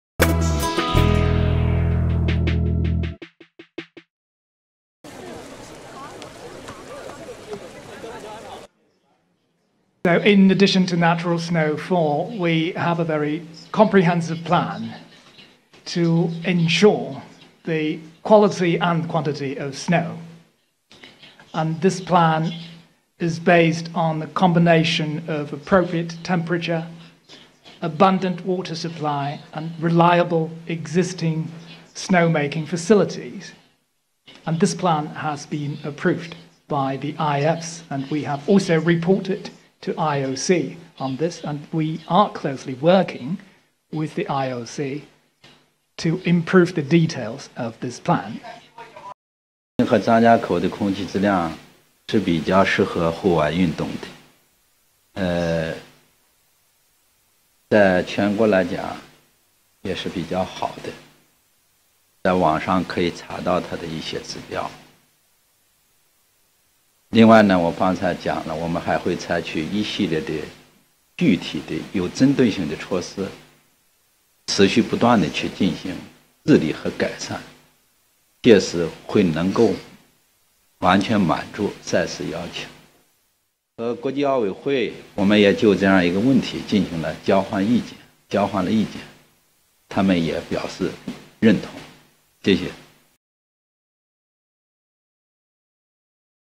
Beijing confident after IOC presentation
The Beijing team bidding for the 2022 Winter Olympic Games has dismissed concerns over snow conditions and air quality after presenting their case to the IOC.
Beijing and rival city Almaty in Kazakhstan met IOC delegates in Lausanne, Switzerland on Tuesday.